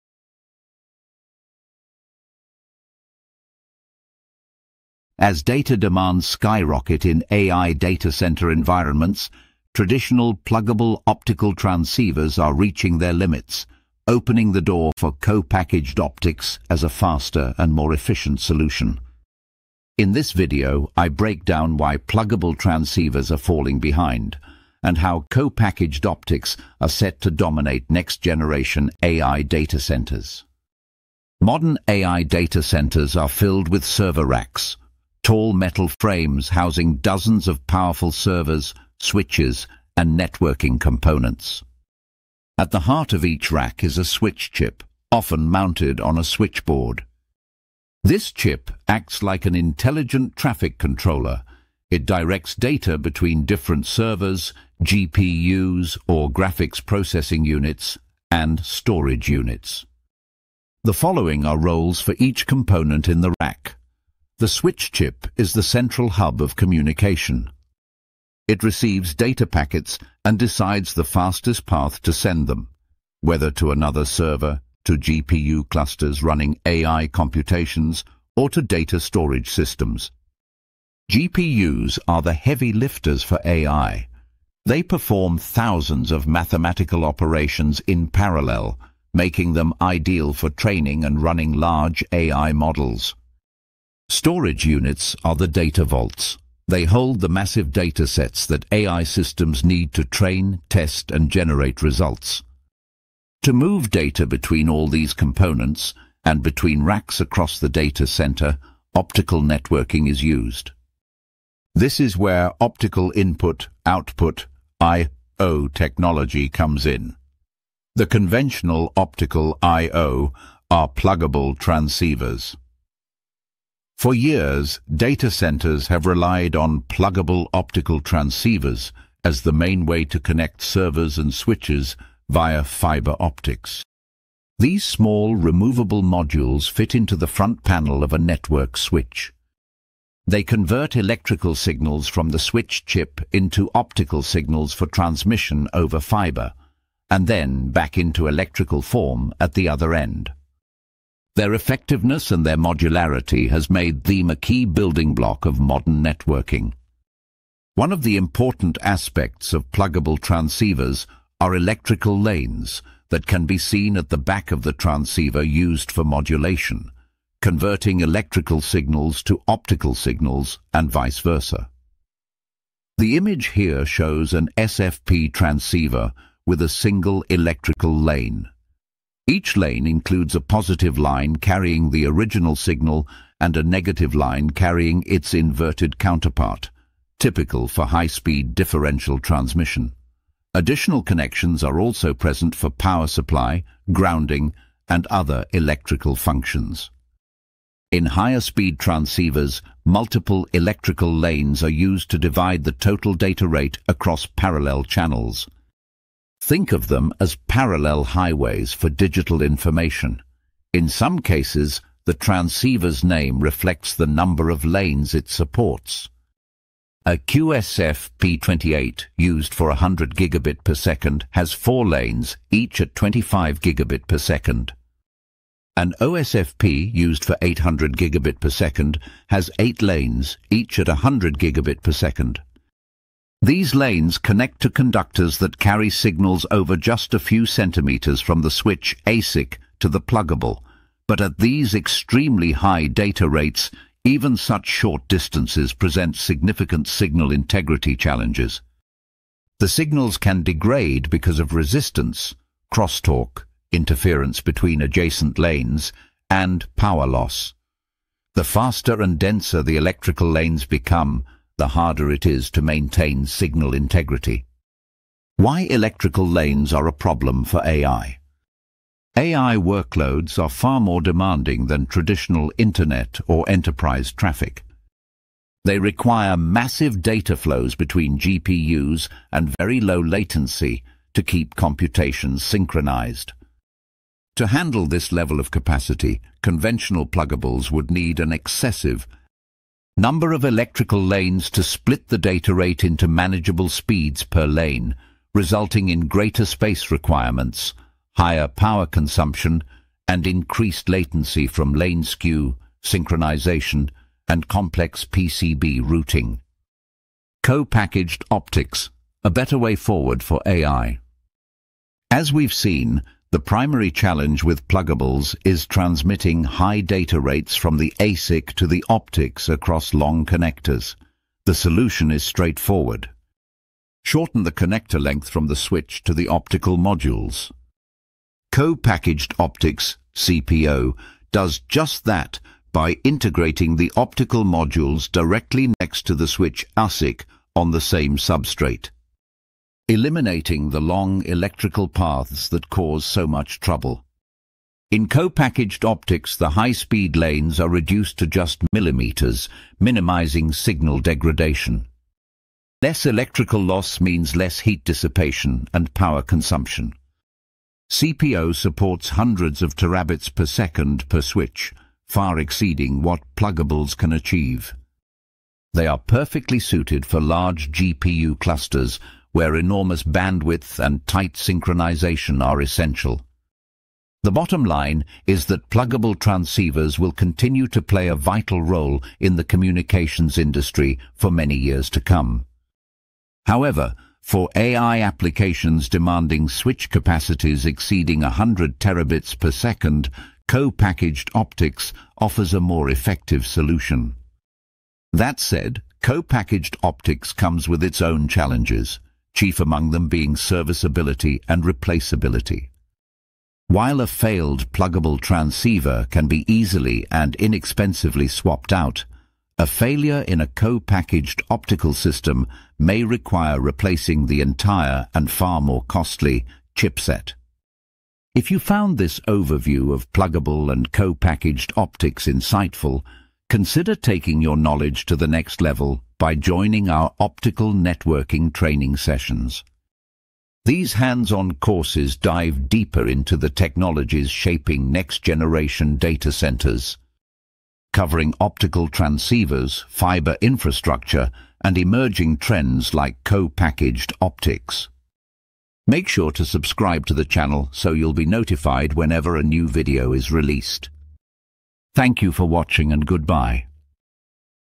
Co-Packaged Optics: Why Traditional Pluggable Transceivers Are Struggling to Keep Up With AI
As data centers continue to scale in response to the explosive growth of AI, cloud computing, machine learning, and high-performance applications, traditional pluggable optical transceivers are beginning to show their limits. For years, pluggables have been the backbone of data center interconnects, offering flexibility, interoperability, and ease of replacement. But as we push into 800G, 1.6T, and beyond, the fundamental challenges of moving extremely high-speed electrical signals across a circuit board are becoming impossible to ignore.

In this video, we take a deep dive into the bottlenecks facing today’s pluggable optics and explain why the industry is increasingly shifting toward Co-Packaged Optics (CPO) as a long-term solution. You’ll gain a clear understanding of how electrical and optical technologies interact inside modern switches—and why next-generation architectures are needed to keep pace with the data demands of AI-driven workloads.

Here’s what you’ll learn:

• The limits of high-speed electrical lanes
We explain how switch ASICs send data to pluggable optics through electrical traces, and why these paths become lossy, power-hungry, and inefficient as speeds reach 100G electrical lanes and higher. You’ll see how signal integrity, latency, and heat dissipation become major barriers to continued scaling.

• Why pluggable optics struggle at ultra-high data rates
Pluggables must sit at the edge of the switch for accessibility, but the distance between the ASIC and the pluggable creates electrical challenges that require expensive DSPs, retimers, and equalization—driving up cost, power, and complexity.

• What Co-Packaged Optics (CPO) is and how it works
CPO places optical engines or chiplets directly next to the switch ASIC, drastically shortening the electrical path. This reduces power consumption, lowers heat, improves signal quality, and enables higher bandwidth density—key advantages for AI data centers and hyperscale networks.

• Optical engines, chiplets, and next-generation interconnects
You’ll learn how innovative designs integrate lasers, modulators, detectors, and drivers into compact modules co-located with the switch silicon. We break down the key components and how they work together to deliver cleaner, faster optical signals.

• The benefits and challenges of adopting CPO
While CPO offers major improvements—lower power, higher density, and better performance—it also introduces new challenges such as thermal management, reliability, supply chain coordination, and maintenance considerations. We cover both sides to give you a realistic view of CPO deployment.

Whether you’re a network engineer, optical designer, datacenter architect, or simply a tech enthusiast, this video provides a clear, accessible explanation of why Co-Packaged Optics is becoming one of the most important innovations in the future of high-speed optical networking. Get ready to understand how tomorrow’s AI-driven data centers will be built—and why pluggable optics alone can’t get us there.

0:00 - Introduction
0:31 - Introduction to modern data centers
1:51 - Pluggable optical transceivers and their challenges
4:25 - Why are lanes a problem for AI?
5:07 - Co-packaged optics, a better way forward for AI
6:14 - Summary
6:58 - Enhance your knowledge of optical communications with certifications from OTT